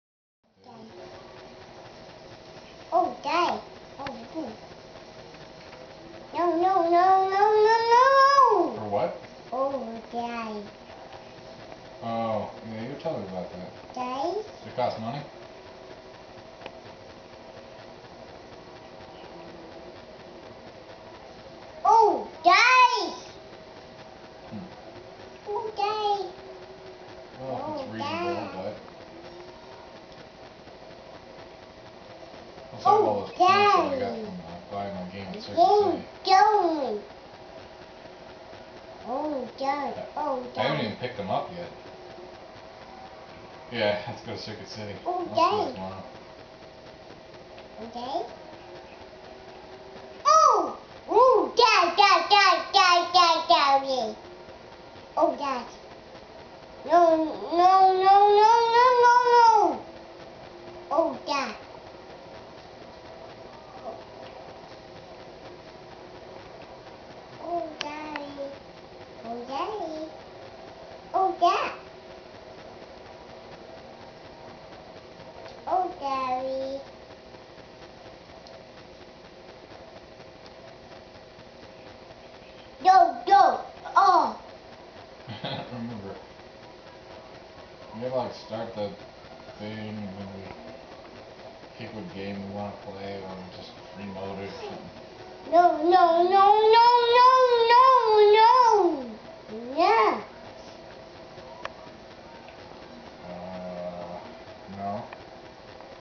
video game fan
Matthew cheers on his daddy as he races cars on a video game